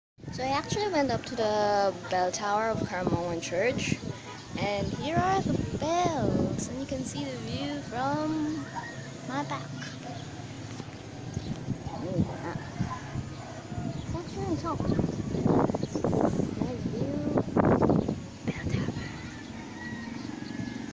Caramoan Getaway (Day 3): Finding the Bell Tower
We're off to Camarines Sur for some 3-day fun in the sun! You can read my full experience at my blog here at: